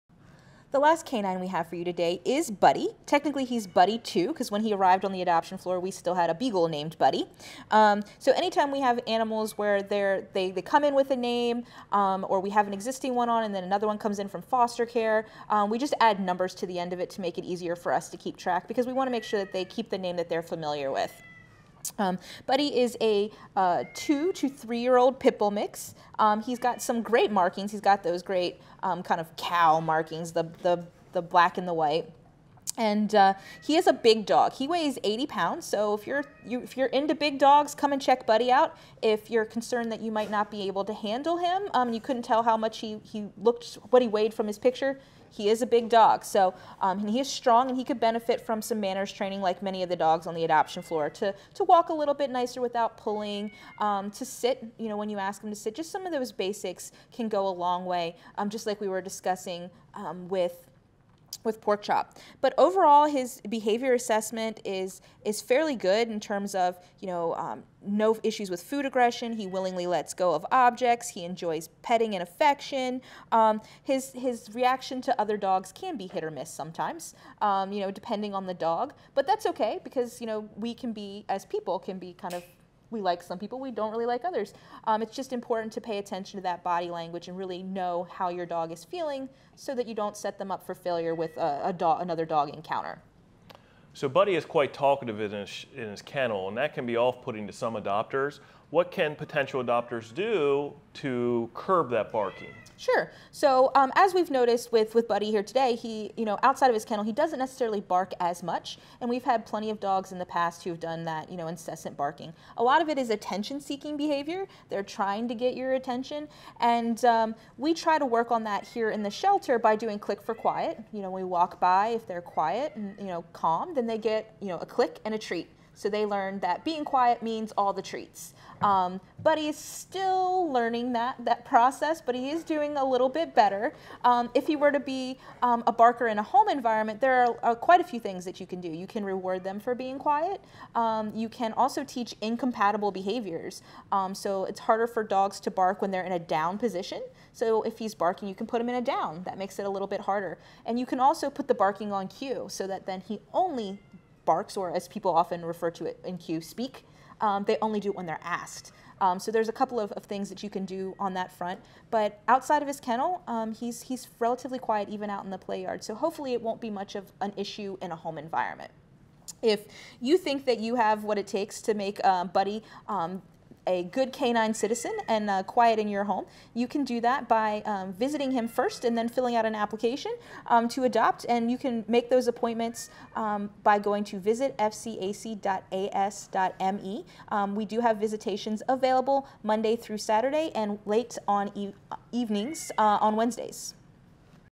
Buddy 2 - 230464
Meet Buddy 2, a Terrier, American Pit Bull / Mix, Age: 3y, Gender: Male, Color: Black / White